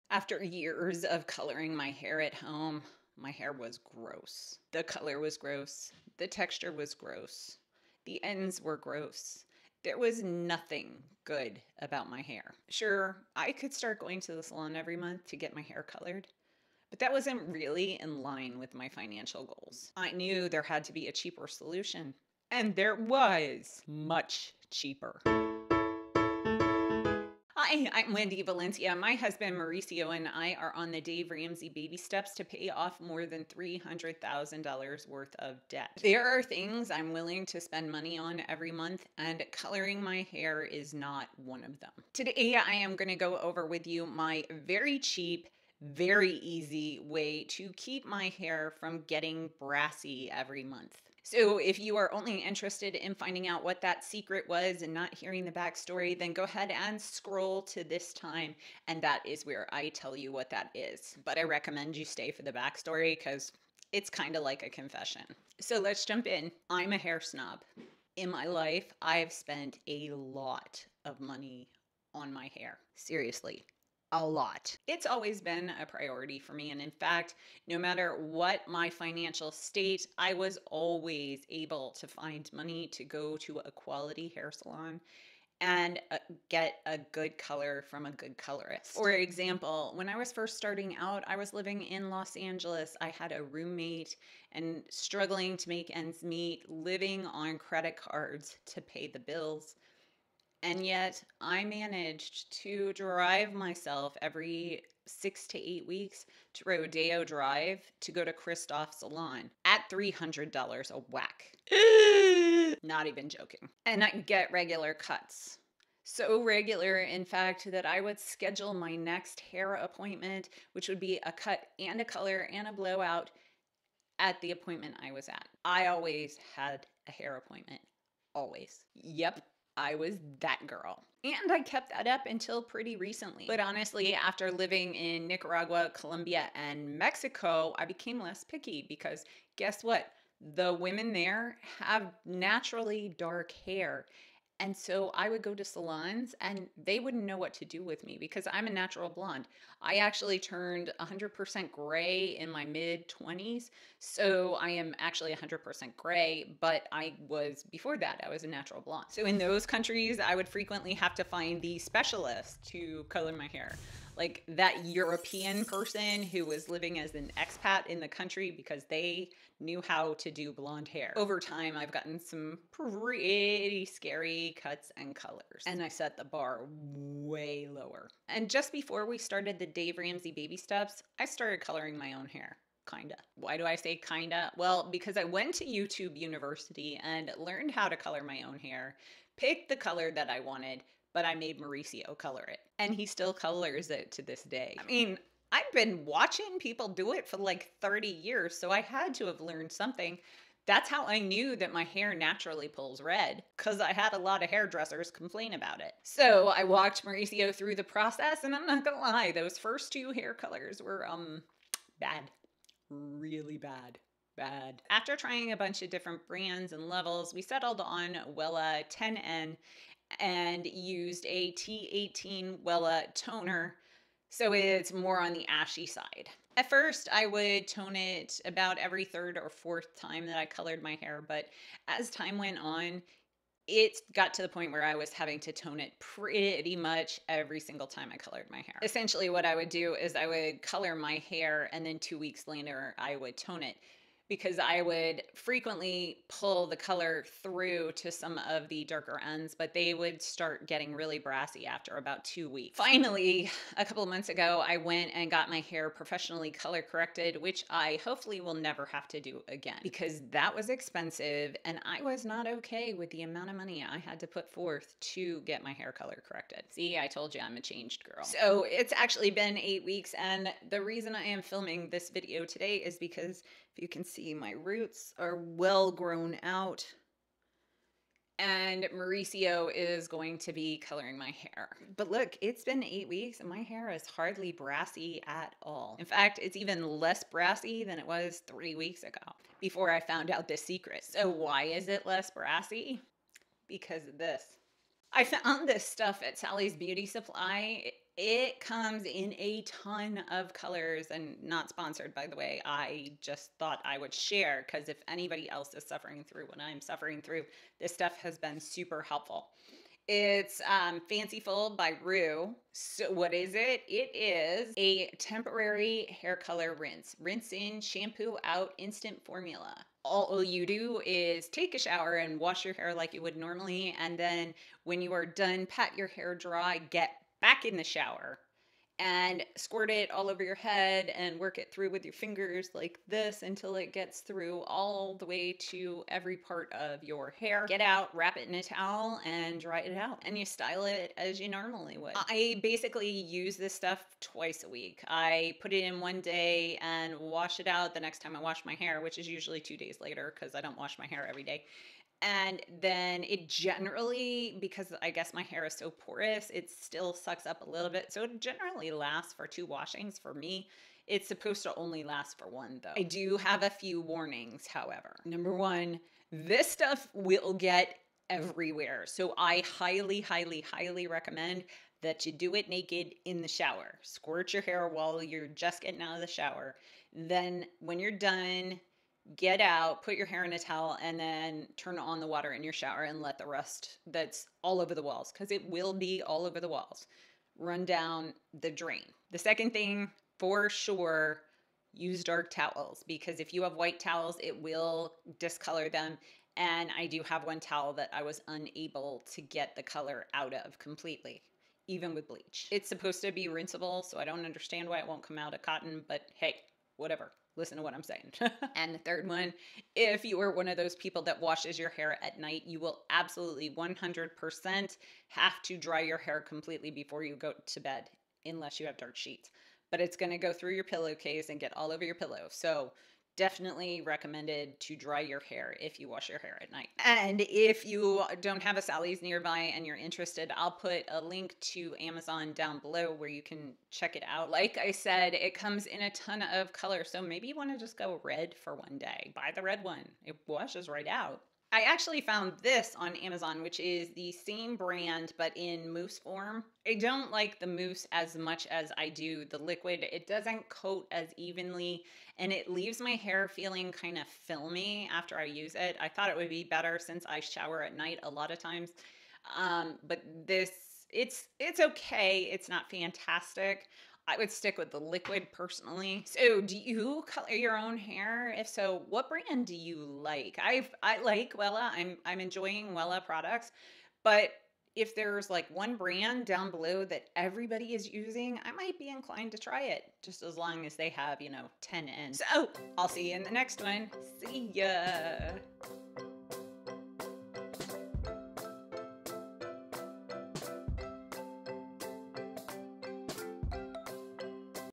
Brassy Hair Fix (Super Cheap!!)
I found a SUPER CHEAP brassy hair fix! I was having such problems with brassy platinum blonde hair. I use Wella 10N (20 volume developer) and Wella T18 toner every month yet I still had brassy hair. But I finally found way to fix it!

NOTE: EVERYTHING IS WAY CHEAPER AT SALLY'S BEAUTY SUPPLY!